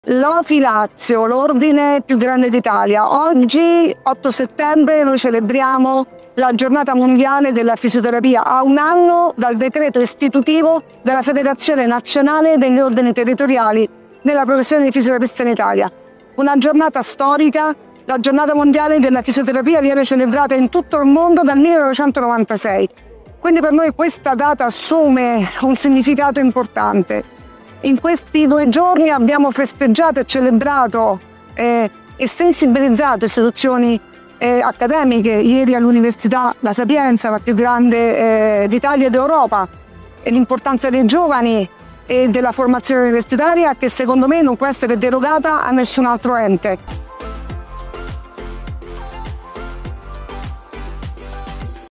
Giornata mondiale della fisioterapia
Abbiamo celebrato la Giornata mondiale della fisioterapia con ofilazio l'8 settembre nel cuore di Roma, Piazza del Popolo un gazebo per farsi conoscere dai cittadini